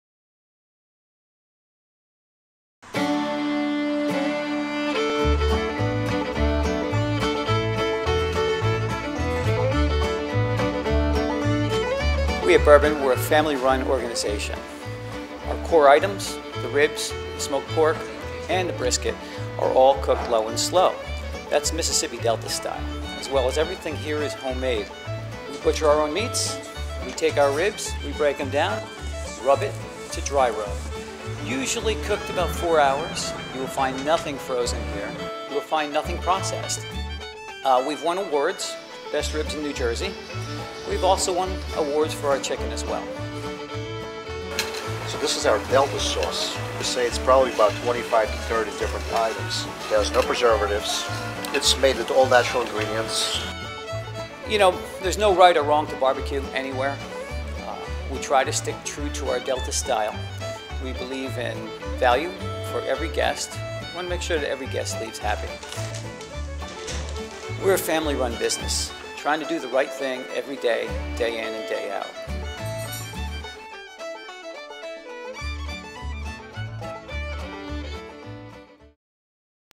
Bourbon BBQ & Catering introduction
Bourbon BBQ is an Authentic Southern all Hickory Barbecue & Catering, and is a Family Owned full service restaurant. It was named the "Best BBQ in New Jersey" by NJ Monthly. Open 7 days a week for Lunch and Dinner, Bourbon BBQ is a BYOB family restaurant that offers Authentic Southern Fried Chicken, BBQ Ribs, Brisket, hand-pressed Hamburgers, Chili and other local favorites. Simple food done right.